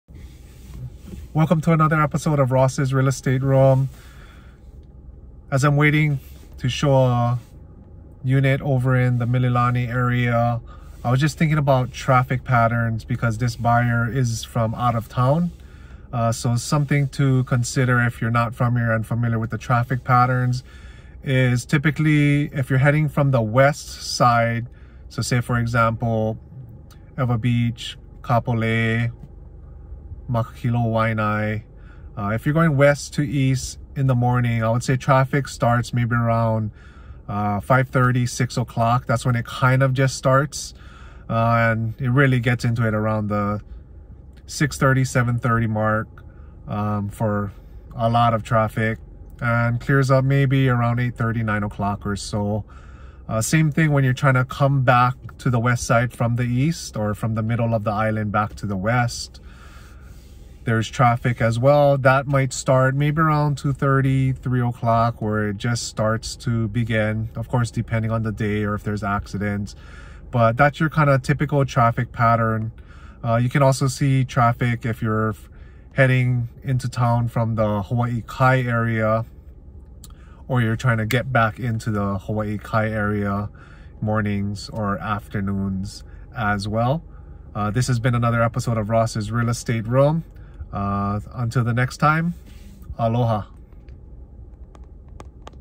Traffic Patterns Oahu | Buying and Selling Hawaii Real Estate on Oahu
Basic traffic Patterns to know about in Buying and Selling Hawaii Real Estate on Oahu.  Great to know for buyers not familiar with Oahu.

FREE BOOK FOR SELLERS - Guide to Selling Your Hawaii Home For Top Dollar

FREE BOOK FOR BUYERS - Complete Guide To Buying A Home